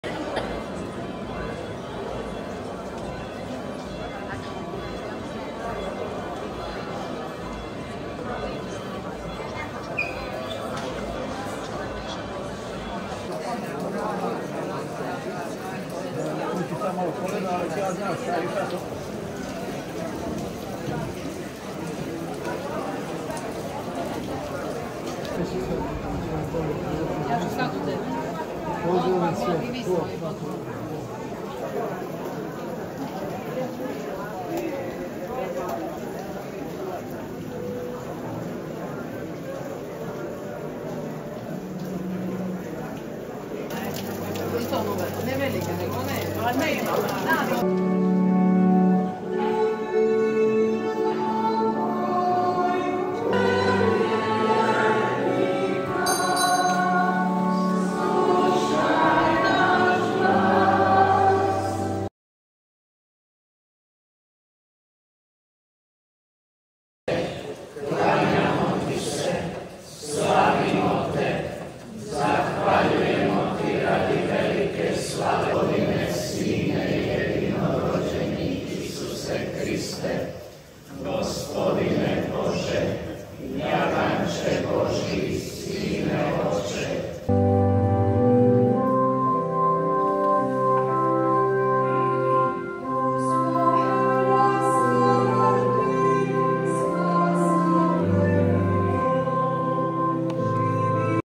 Blagdan sv. Ante na Poljudu
U samostanu sv. Ante na Poljudu služena je sveta misa na blagdan sv. Ante.